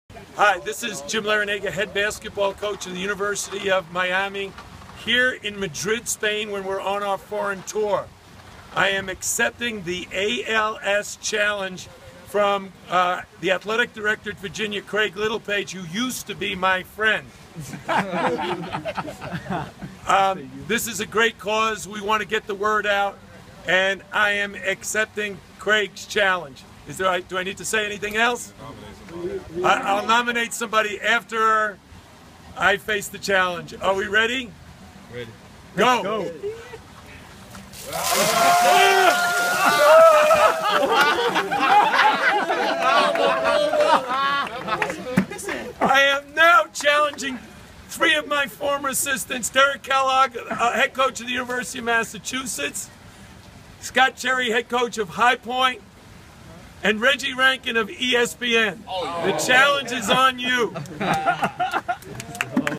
Coach Larrañaga - ALS Ice Bucket Challenge
@CanesCoachL took the ALS ice bucket challenge, and now he challenges former assistants UMass' @CoachKellogg, HPU's @Coach11Cherry & ESPN's @ReggieRankin